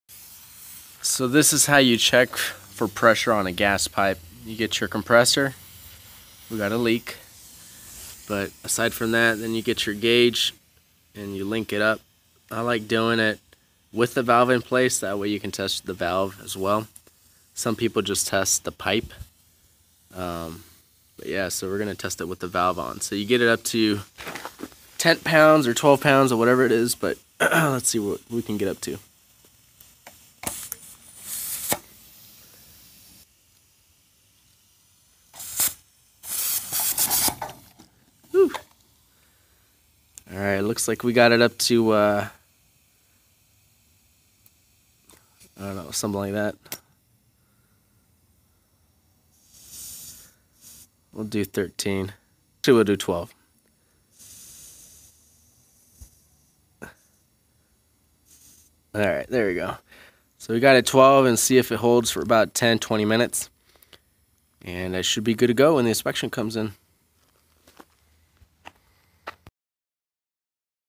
How To Test A Gas Line, Pressure Test And Pass Inspection
David Armenta is a real estate agent with Avenida Realty in Tucson, Arizona.
Armentapllc.com

Testing pressure on a gas pipe. How to. Compressor 3gal, air pressure gauge nipples and fittings. Test for 10lb for 15 min.